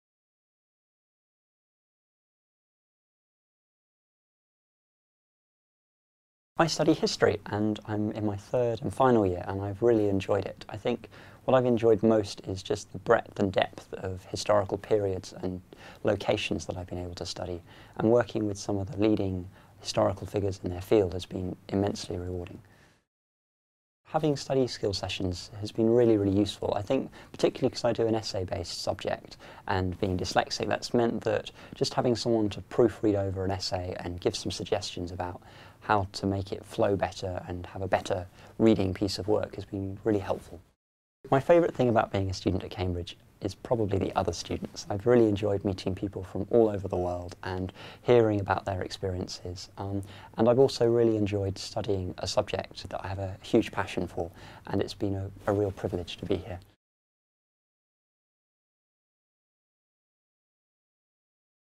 Jono, History - 60 Second Impressions
The '60 Second Impressions' are a series of one-minute films featuring current Cambridge undergraduate students . These students talk about what it's really like to study at Cambridge, live in a College, and take part in a wide range of extra-curricular activities.

Jono is in his final year at Cambridge studying History.  In his 60 Second Impression, he talks about the academic support he has received from various members of staff, meeting other students and his passion for his subject.

The full series of 60 Second Impressions can be found - keep checking back as we'll be adding new films every couple of weeks!